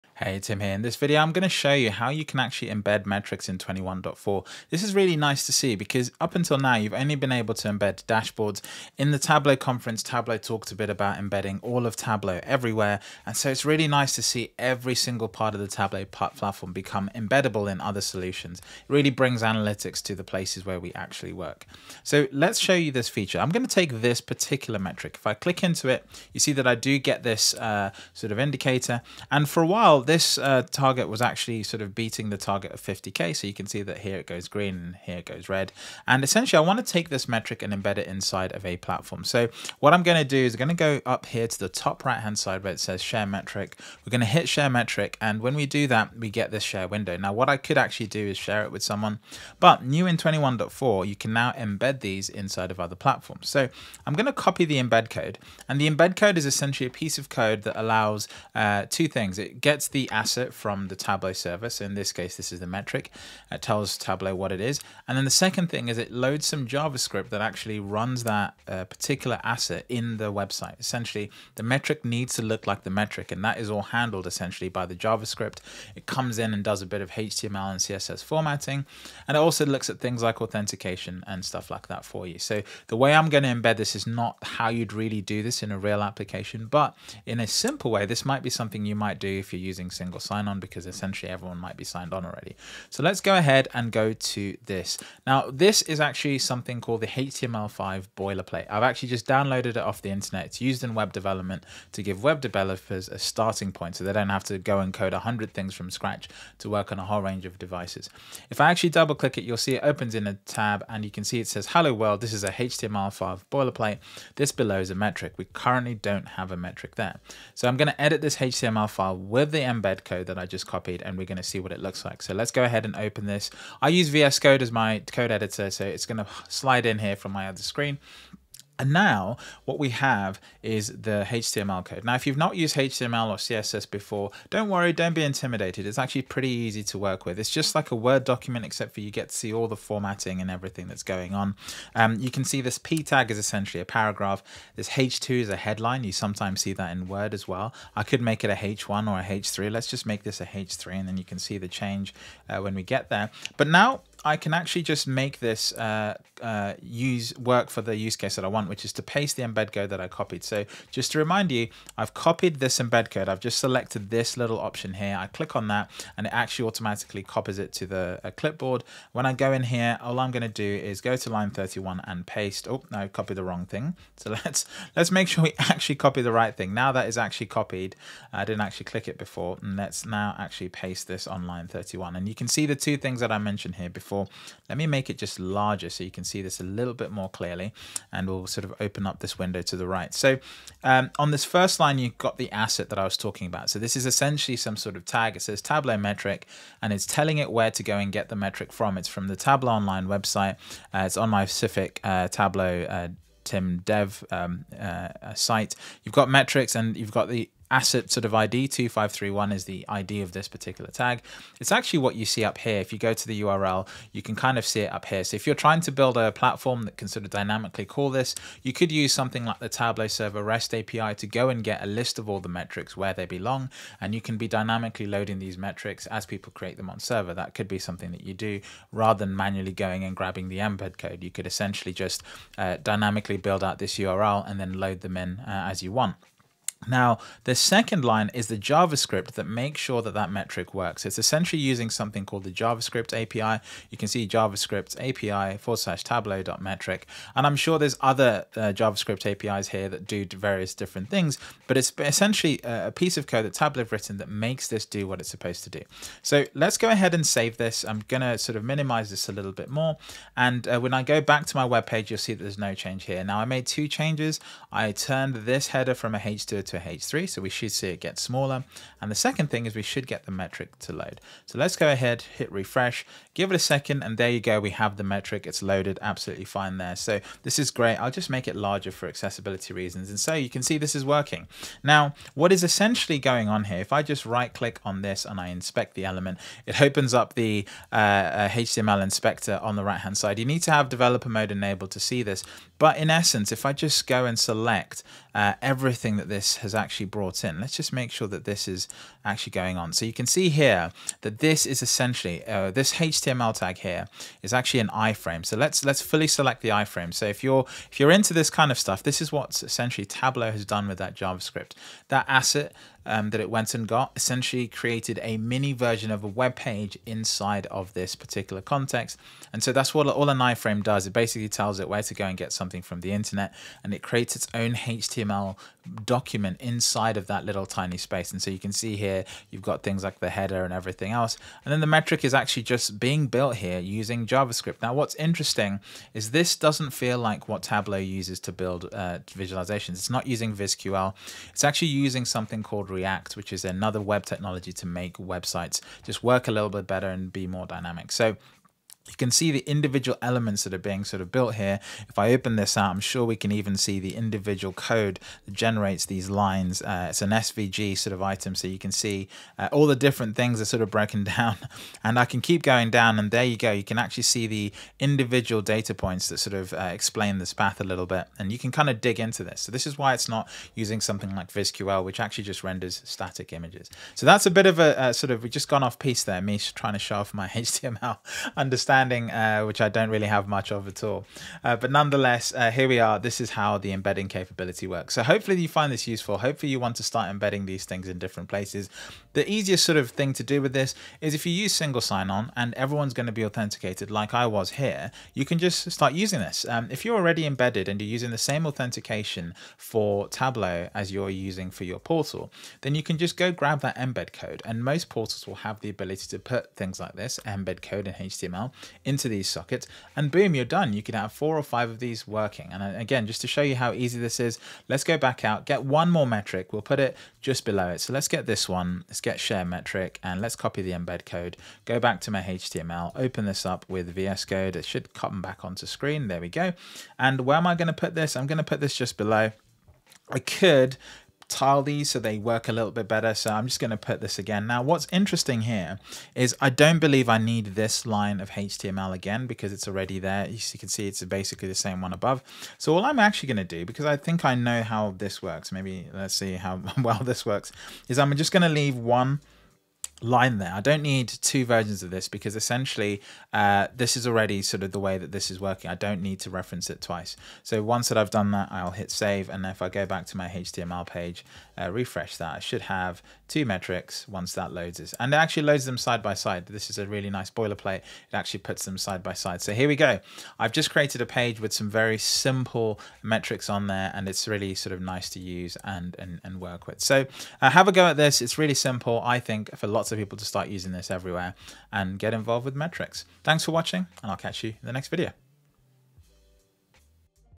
Embed Metrics: New in Tableau 2021.4
Tableau Release notes:
With 2021.4, you can now embed Metrics into your analytical apps, corporate portals or web pages to provide a more personalised experience for different people and teams.